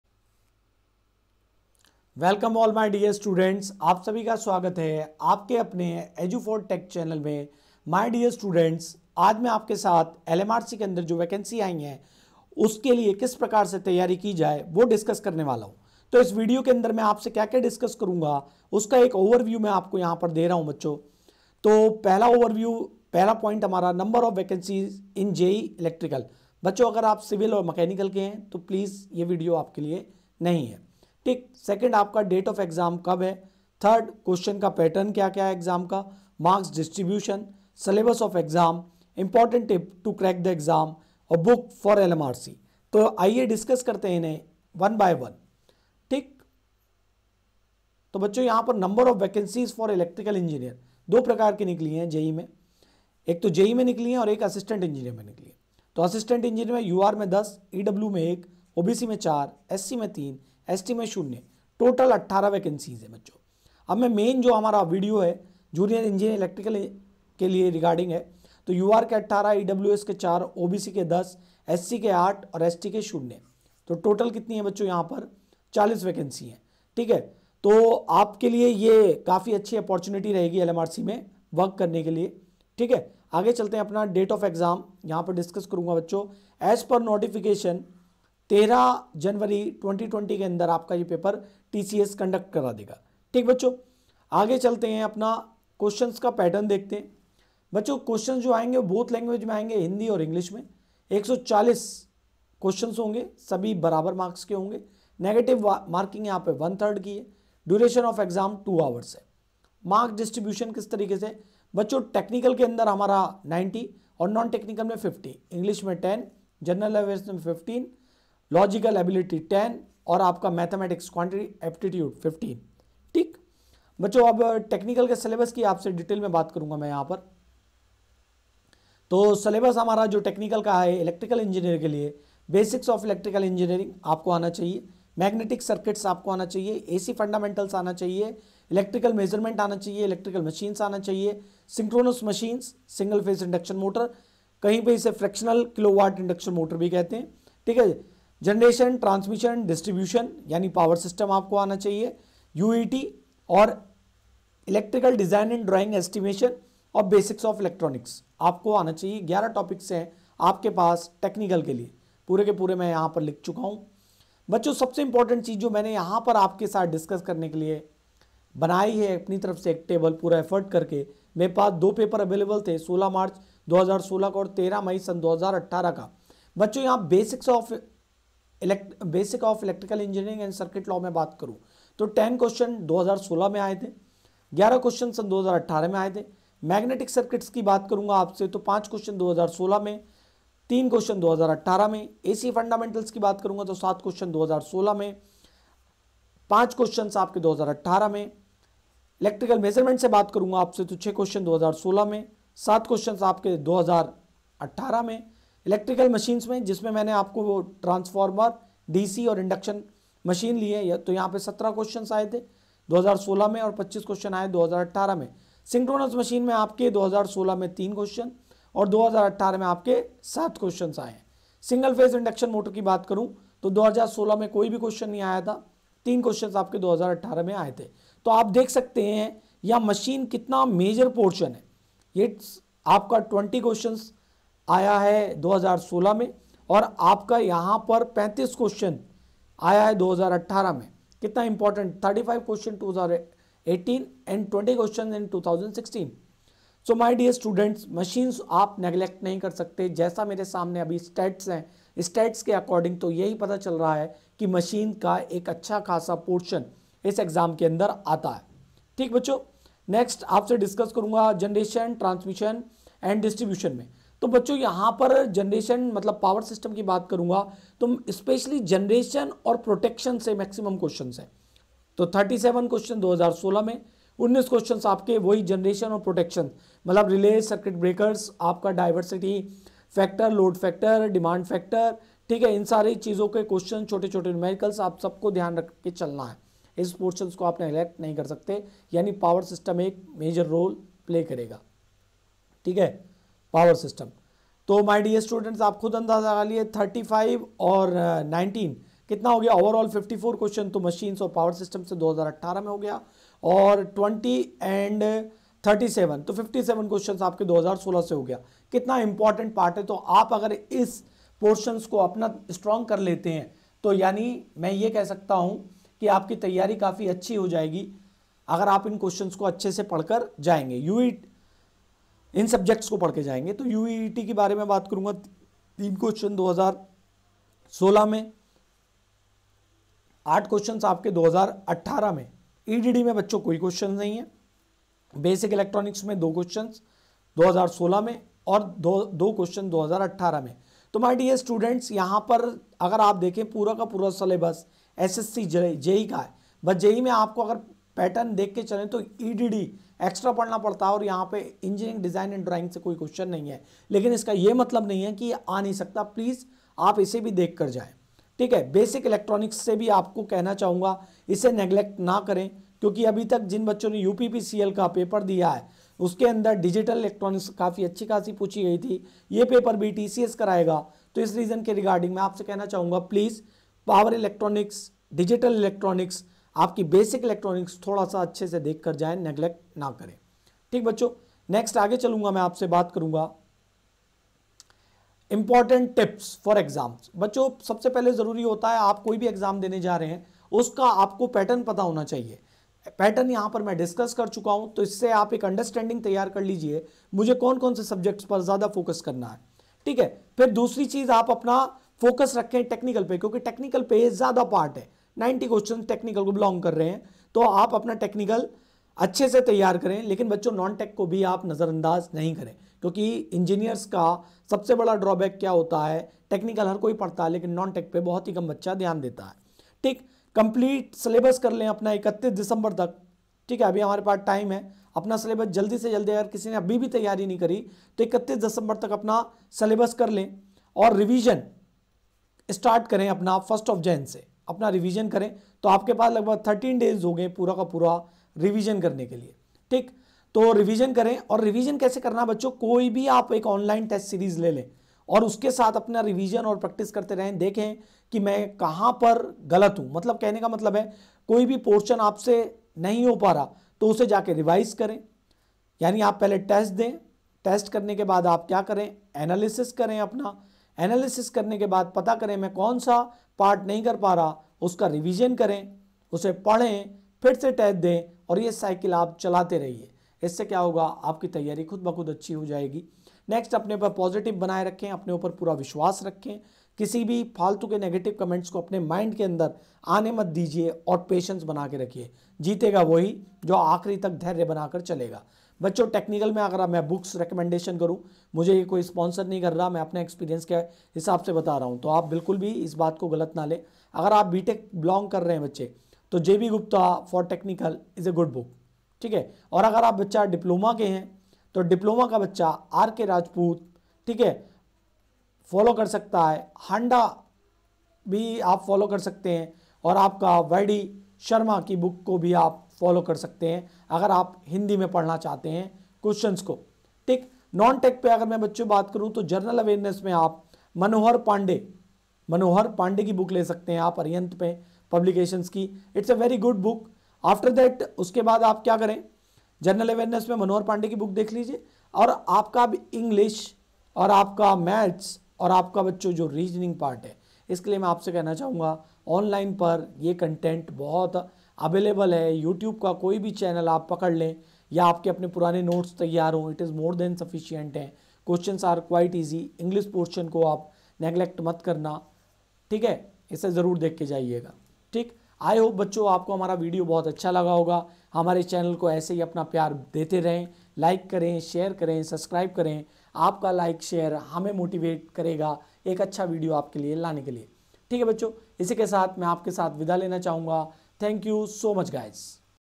LMRC की तैयारी कैसे करे II Syllabus &Important Tips for EE II Rahul sir
In this video I explain the good way to start your preparation for lmrc exam. I also explain the marks distribution lmrc and question pattern. I also give you the exact idea about number of questions come from which portion of syllabus.
Also recommend the important book.